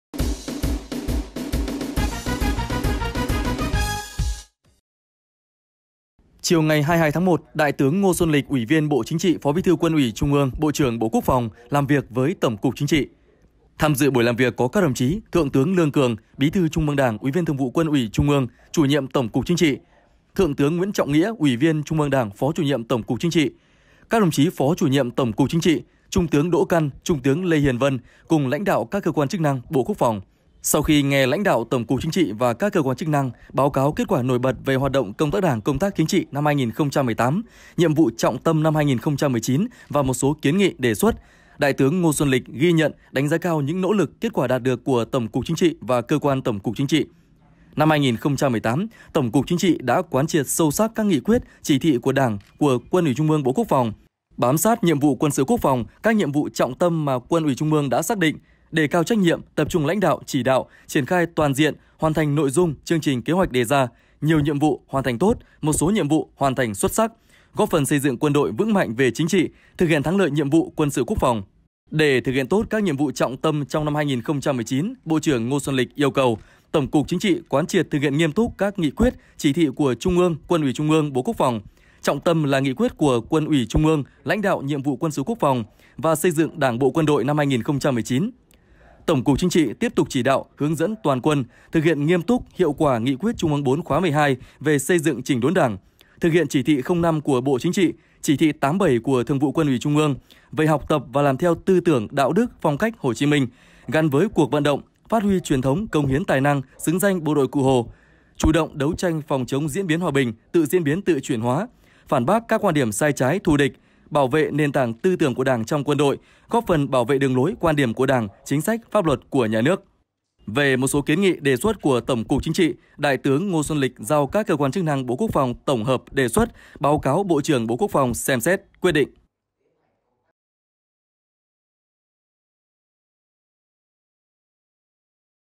Đại tướng Ngô Xuân Lịch làm việc với Tổng cục Chính trị
QĐND Online - Chiều 22-1, Đại tướng Ngô Xuân Lịch, Ủy viên Bộ Chính trị, Phó bí thư Quân ủy Trung ương (QUTƯ), Bộ trưởng Bộ Quốc phòng (BQP) đã làm việc với Tổng cục Chính trị (TCCT).

Âu Lạc - Kênh thông tin chính thống về chính trị, quốc phòng, an ninh...